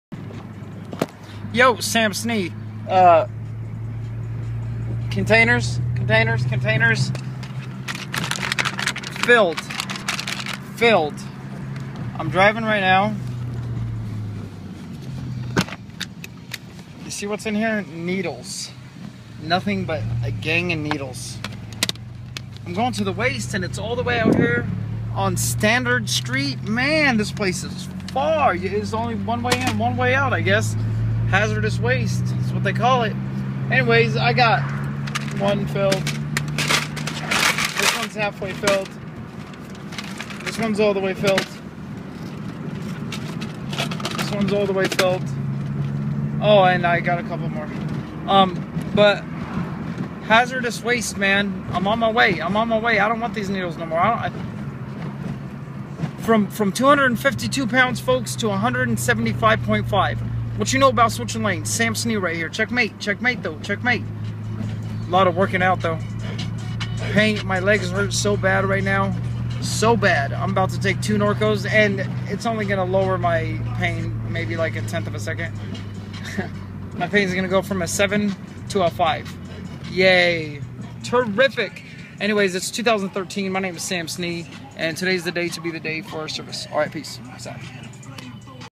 So I'm done Wyth the needles!
Multiple sclerosis if I'm going to be in pain Wyth and wythout so I would rather be Wyth out.